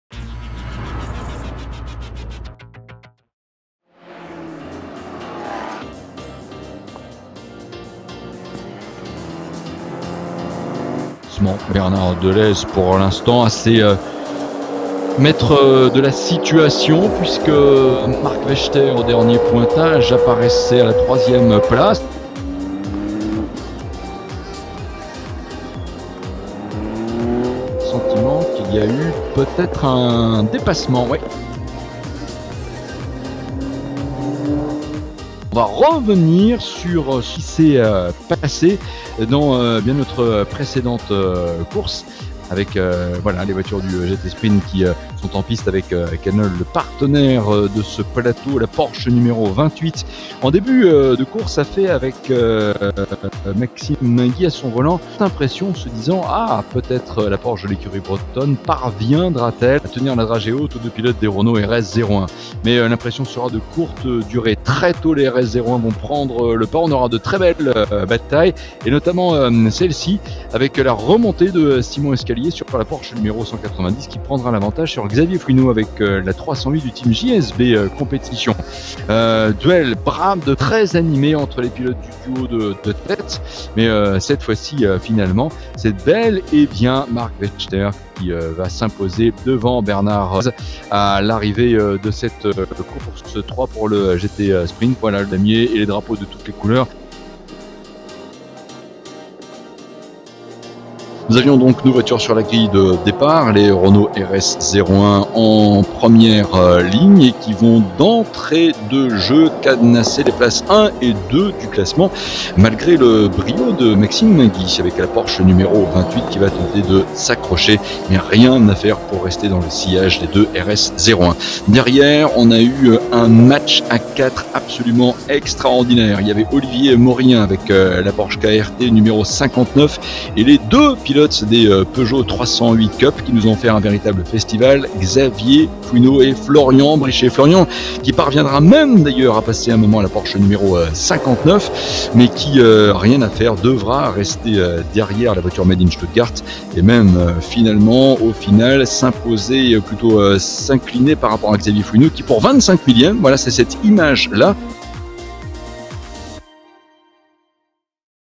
HIGHLIGHT | KENNOL GT-TOURING SPRINT | Paul Ricard
Retour en image sur les premières courses KENNOL GT-TOURING Sprint de cette saison 2021 🏁

Suite à un incident techniques, nous n'avons pas pu obtenir d'images de la course n°1.